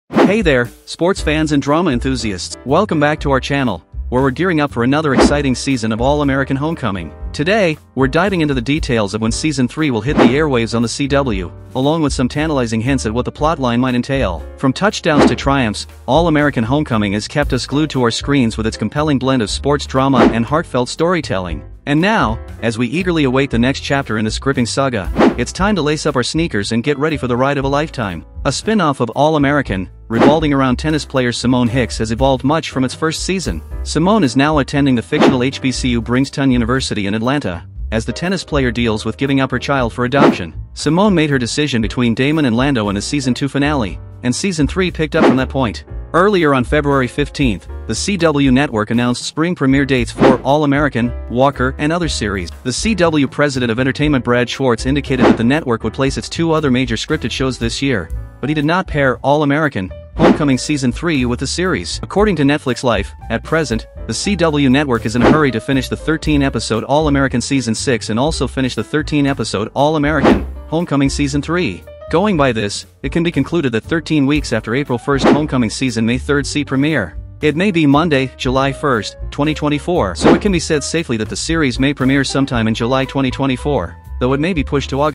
All American Homecoming Season 3 Release Date Update and Preview
Hey there, sports fans and drama enthusiasts! Welcome back to our channel, where we're gearing up for another exciting season of All American Homecoming. today, we're diving into the details of when Season 3 will hit the airwaves on The CW, along with some tantalizing hints at what the plotline might entail.